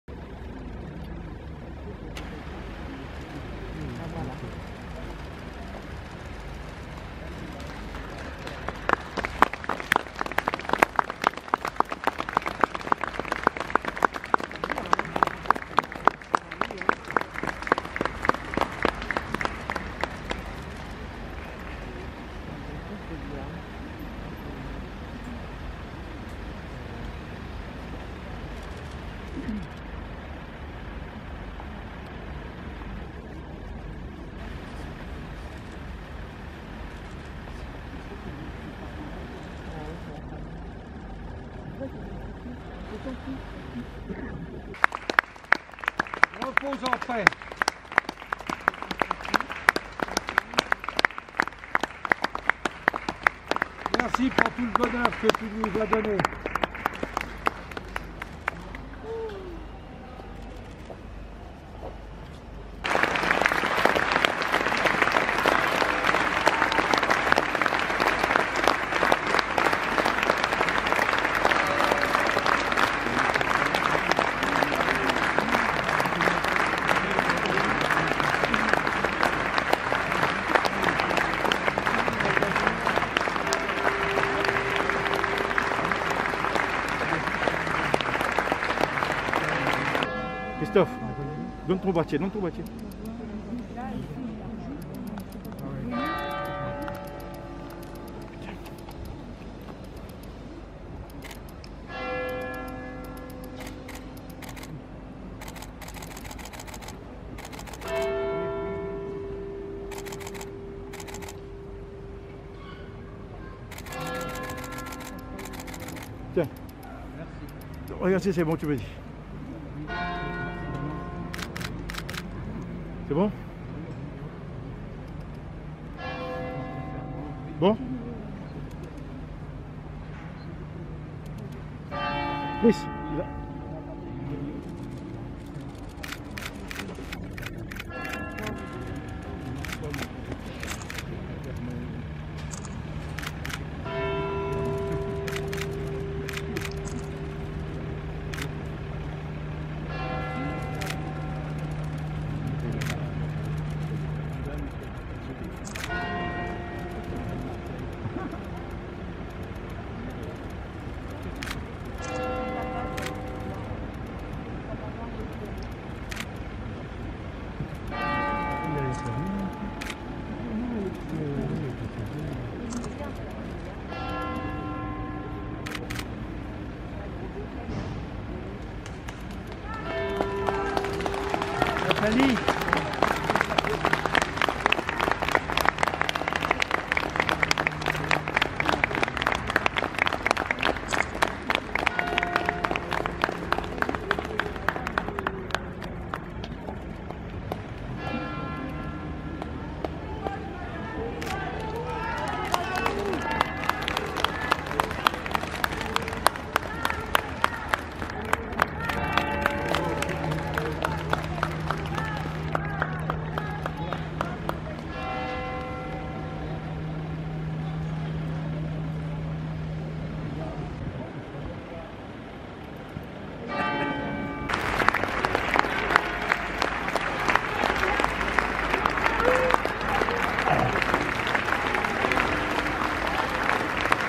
Nathalie Marquay aux Obsèques de Jean-Pierre Pernaut, ses gestes d'affection 😘💋👋pour les badauds
(//‿•) La foule d'anonymes, les badauds, étaient nombreux à lui dire "Courage Madame" auxquel elle a réagit en leur adressant des bisous et des signes de la main ✋très reconnaissante pour tout l'amour pour son mari Jean-Pierre Pernaut, mais aussi pour la bienveillance du public face à sa douleur, avant sa montée dans le mini bus en partance pour Amiens (ou Louveciennes).
De même lors de sa descente de son véhicule, elle a sourit et hoché la tête en signe d'approbation lorsqu'elle fut applaudi dès son arrivée devant le parvis de la Basilique Sainte-Clotilde. Jusqu'à la dernière seconde, assise dans le mini-bus, à travers la vitre passager, elle a salué de la main, la foule rassemblée devant la Basilique Sainte Clotilde à Paris.
Courage Madame Nathalie Marquay, à vous et vos enfants dans cette terrible épreuve.